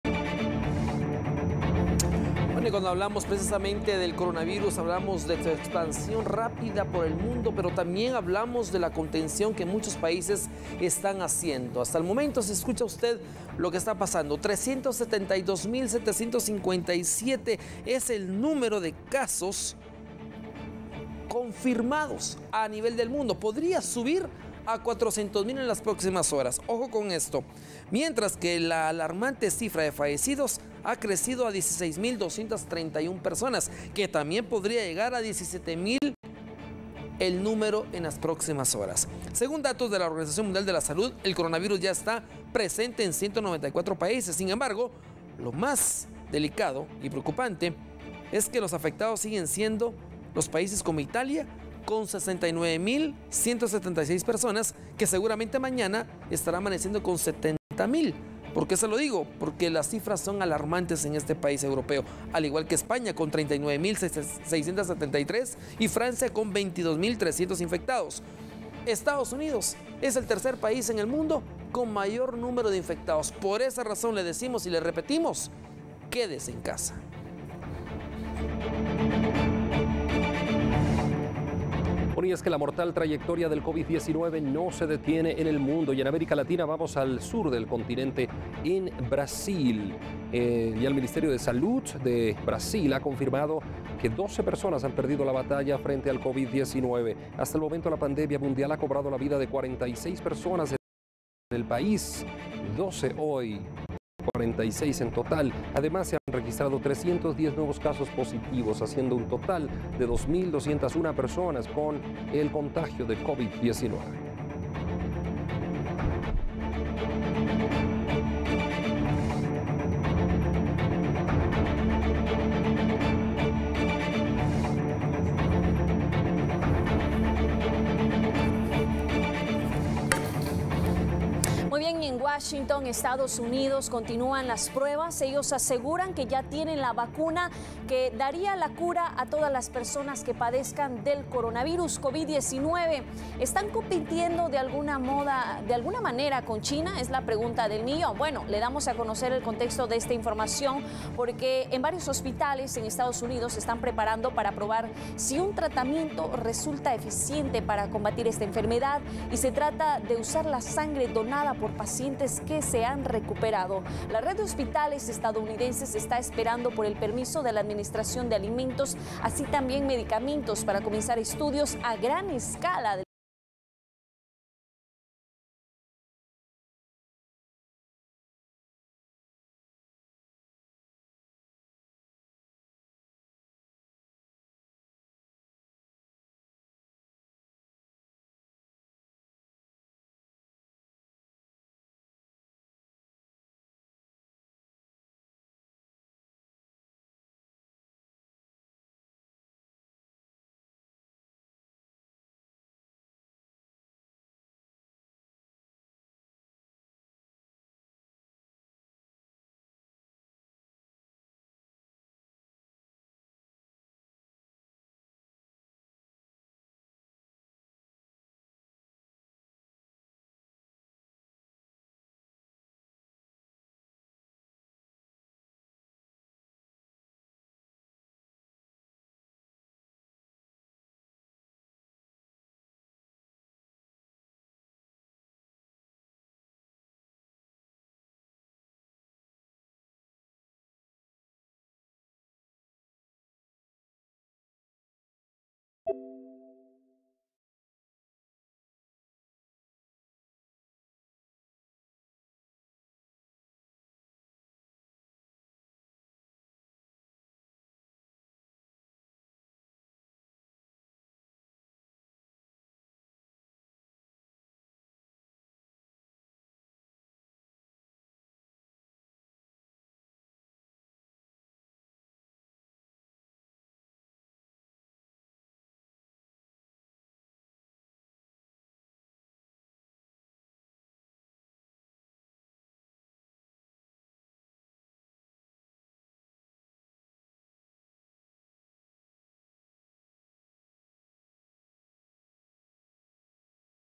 Sexta muerte de Covid-19 en Argentina
Hoy se confirma la sexta muerte debido al Covid-19 en Argentina, dicho país fue uno de los primeros en reportar casos en Latinoamerica.